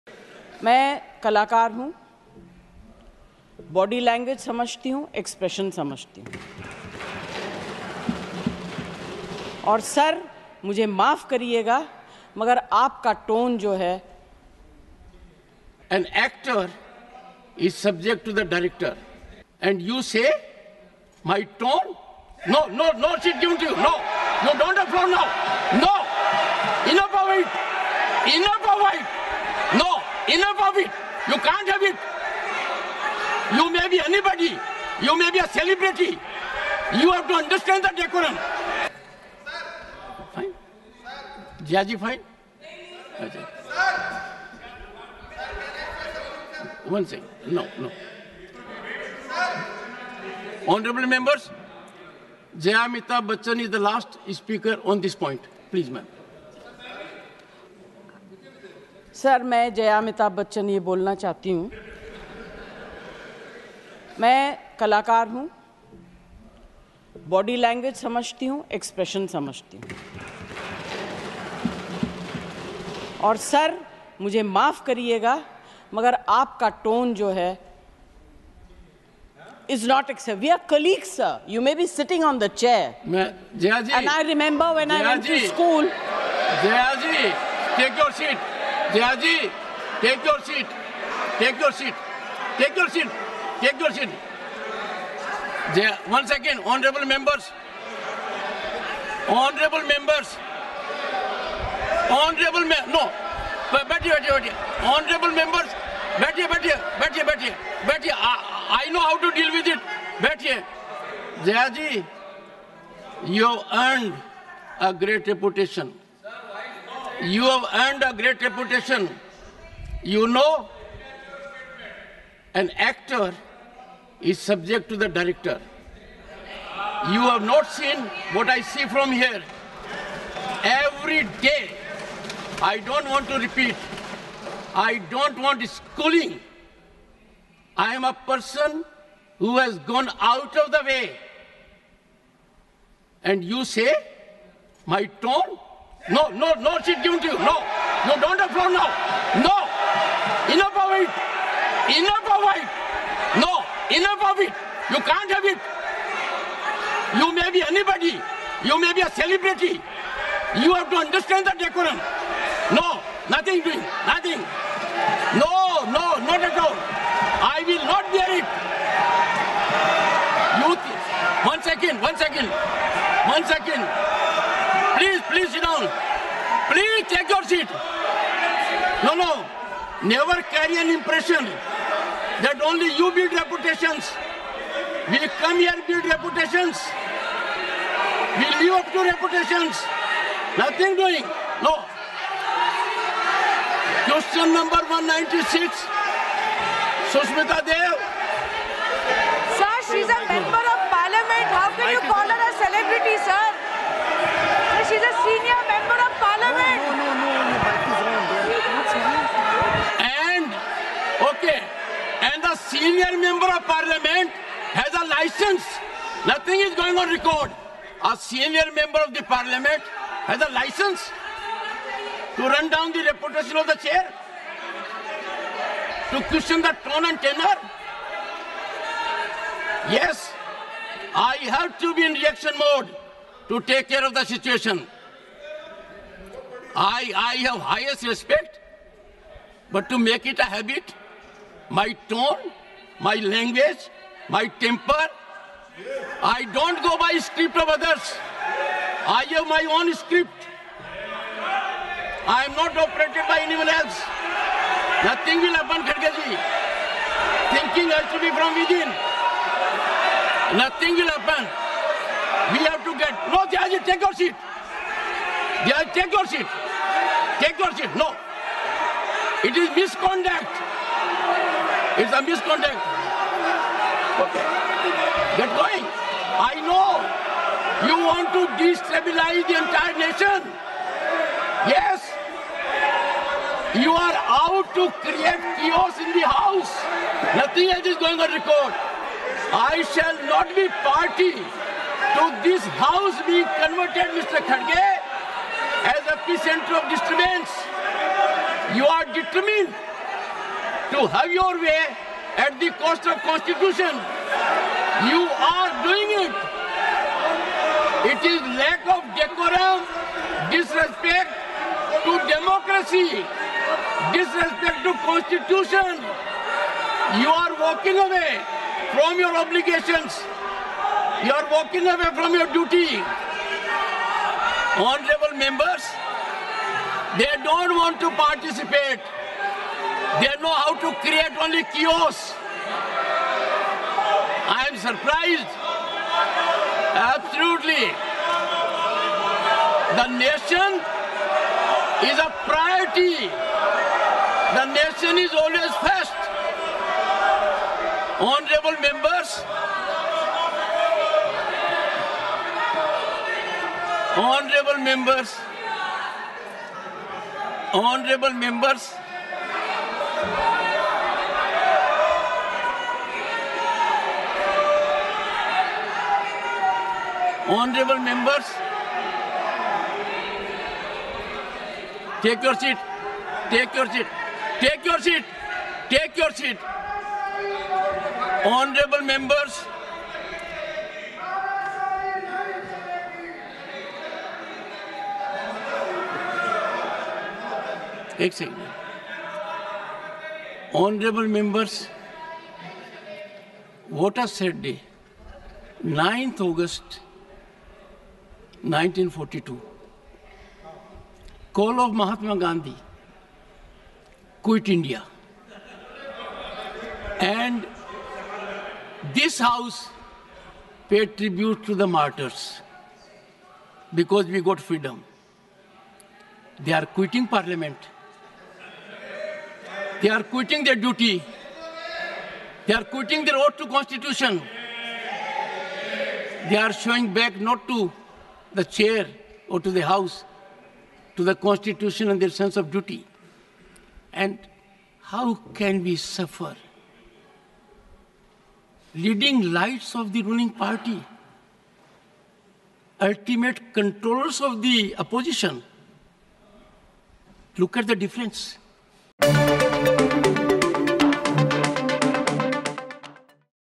Watch: Fiery Jagdeep Dhankhar Vs Jaya Bachchan Clash In Rajya Sabha || Satyapatha News
ପୁଣି Rajyasabha ରେ Jagdeep Dhankhar - Jaya Bachan ମୁହାଁମୁହିଁ || Satyapatha News